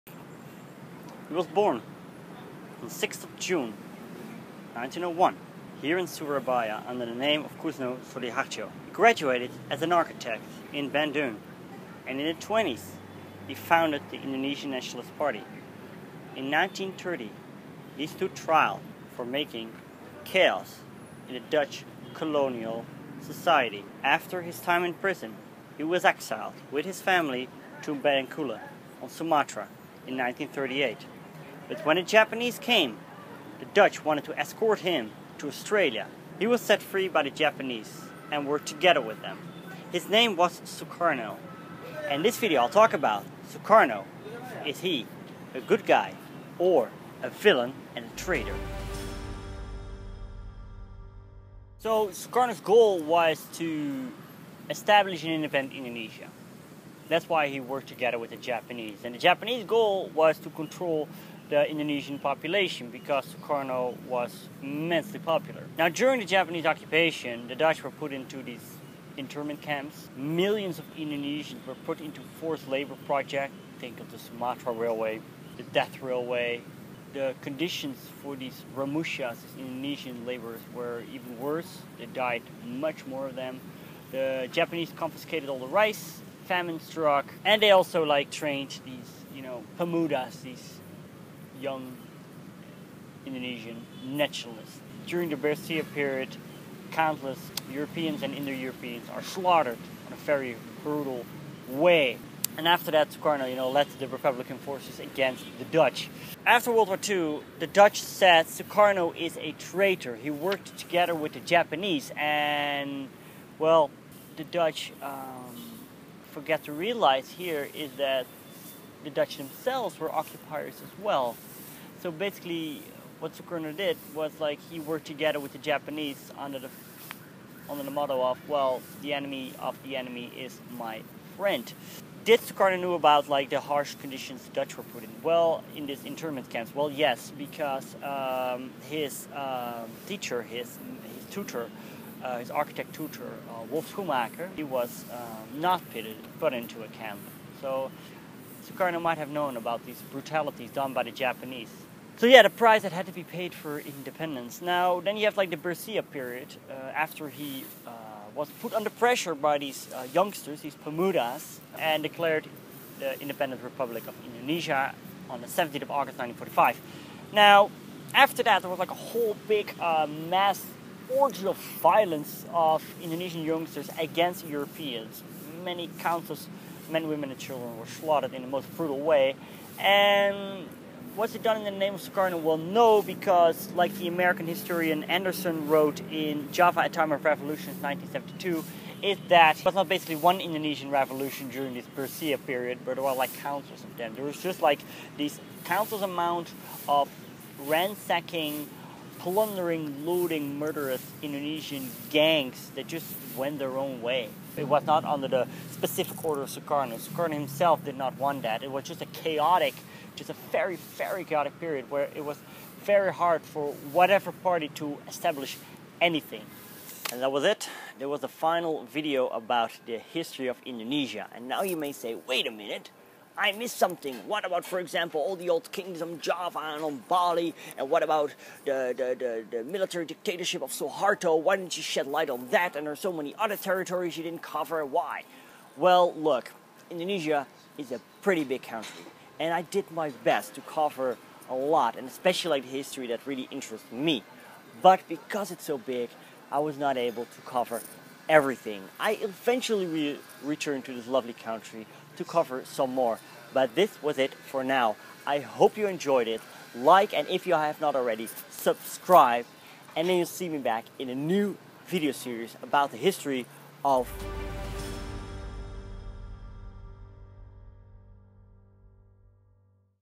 [Indonesia] Sukarno - hero or villain?
Sukarno (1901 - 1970) was the leader of the  Indonesian independence movement that fought agains the Dutch colonizers after the Second World War. In Indonesia they see him as a hero. In the Netherlands he was portrated as a collaborator who worked together with the Japanese occupyer. Who was he?

Recorded on the 6th of June 2016 in Surabaya, Indonesia.

Images from commons.wikimedia.org.